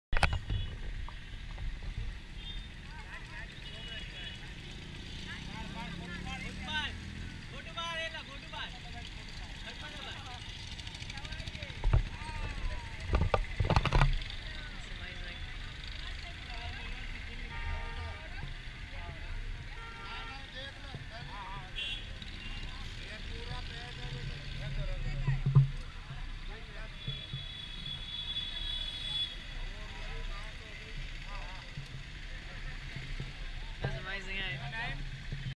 Street Sewing
A lot of business in India is conducted on door steps and in the streets, with many setting up shop where ever there's free space, including on traffic islands like this guy who is sewing up a storm with his hand wound sewing machine! I love India. Such character.